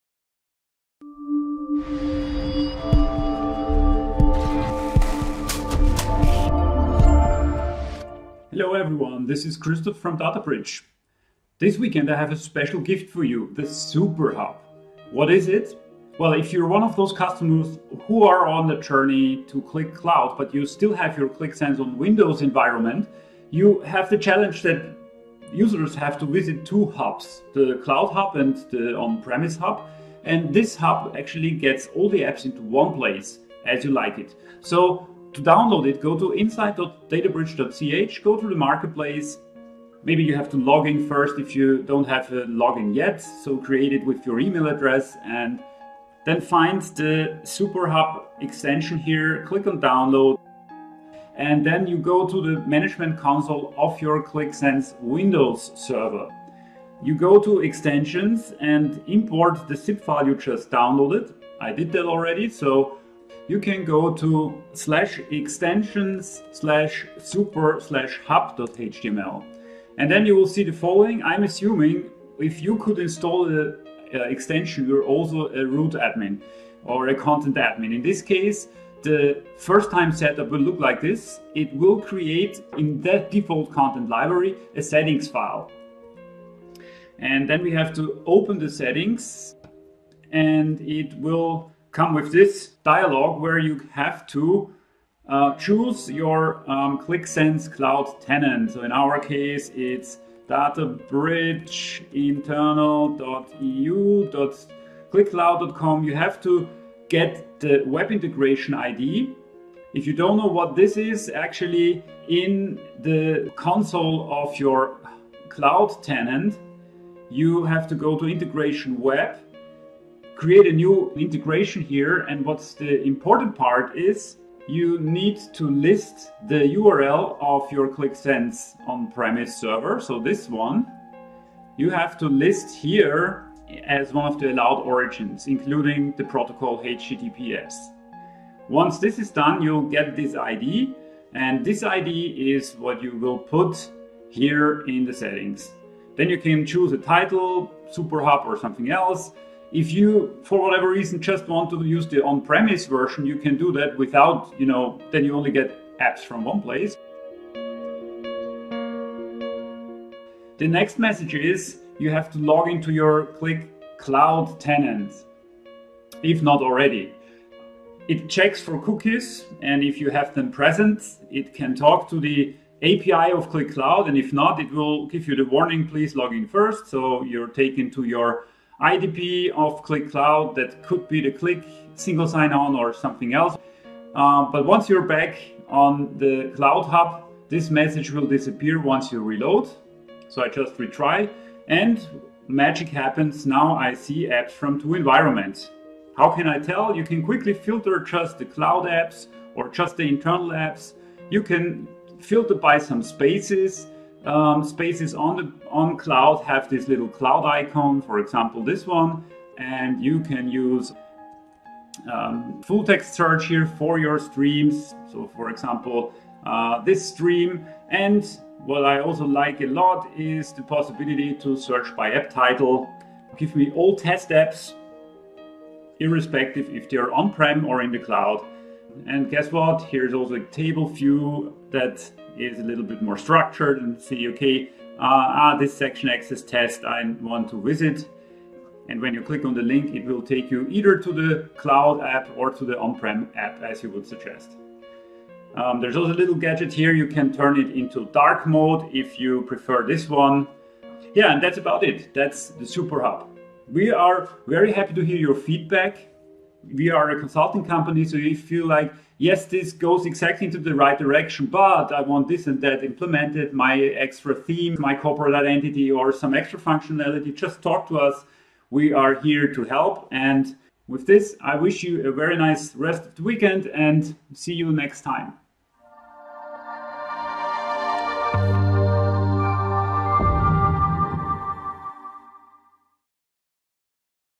Qlik Sense Superhub - free from data/\bridge
Qlik Sense Windows Apps and Qlik Cloud Apps in one place, that's the "superhub", a modern Bootstrap 5 web app.